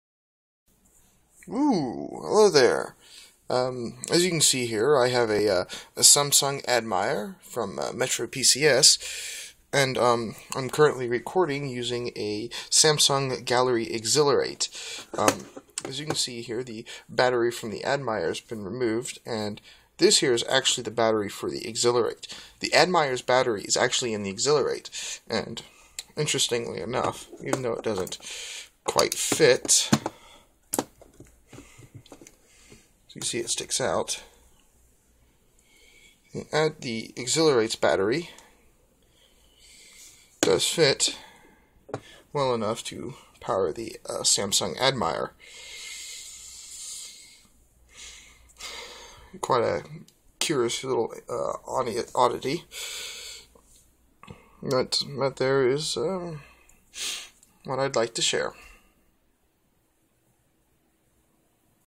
Samsung batteries
Swapping batteries between the Samsung Admire and Galaxy Exhilarate.